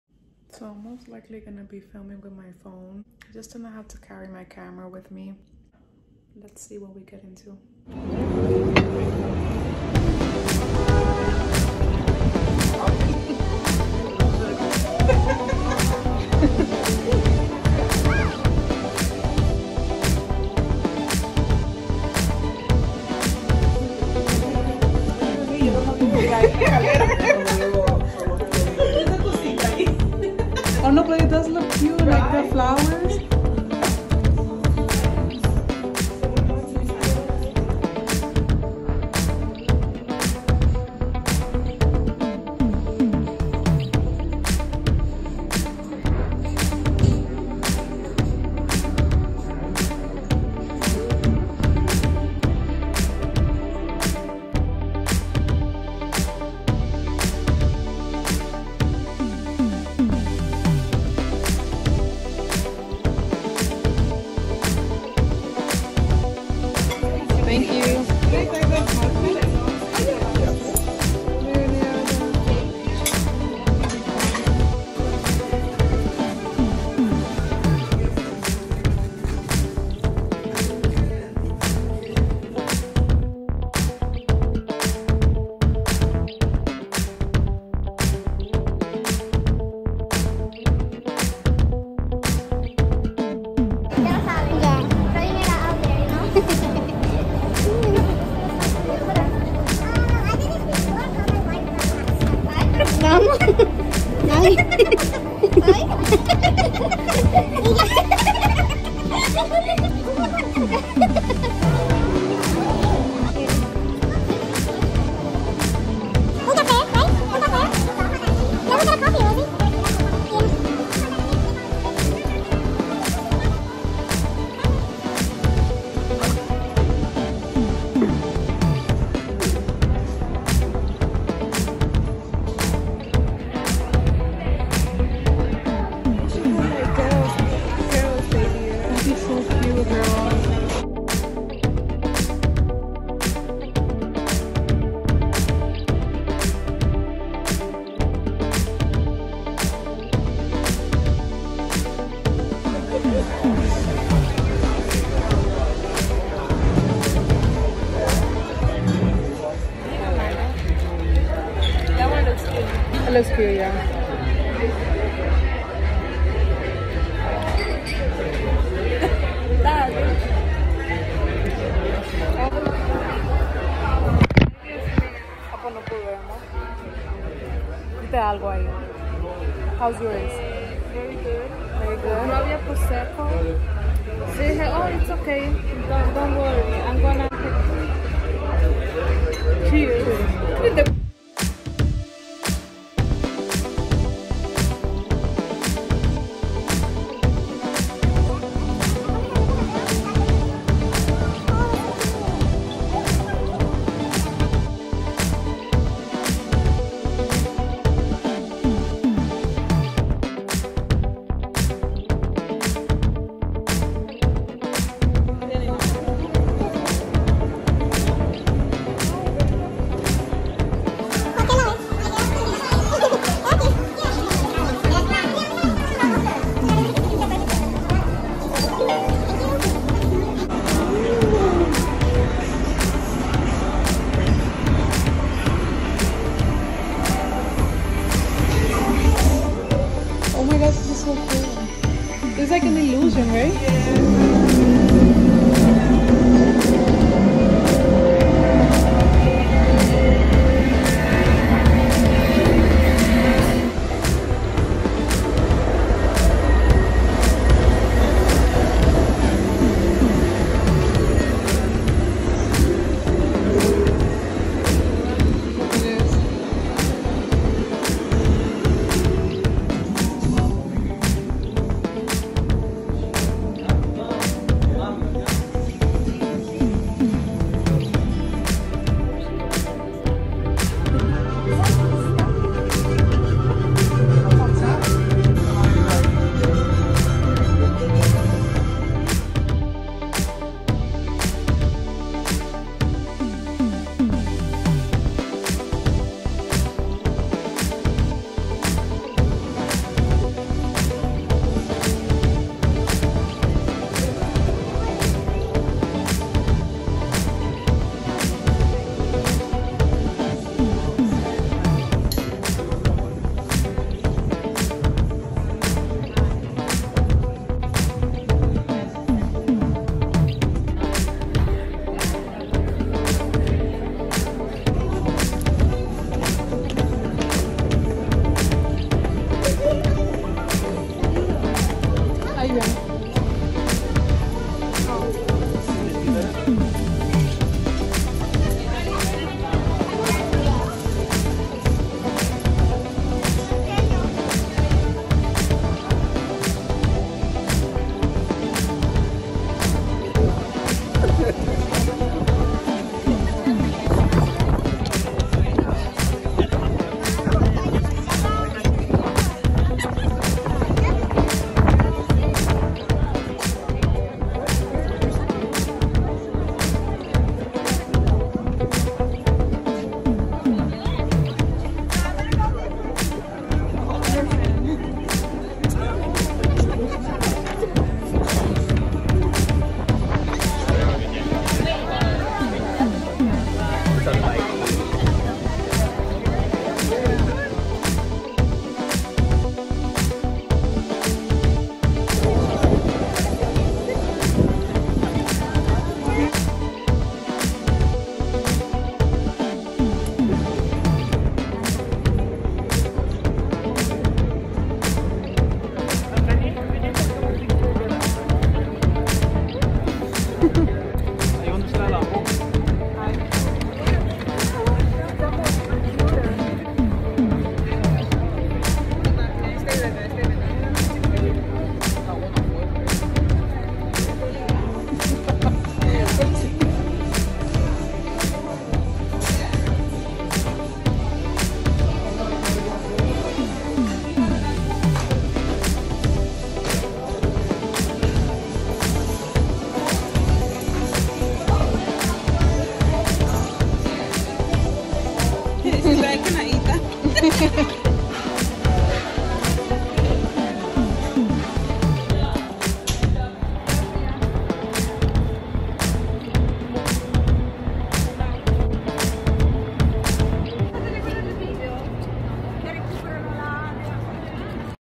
NYC! Dolly Varden, The Edge, Vessel & more
Added some clips of my day in NYC, hope you enjoy!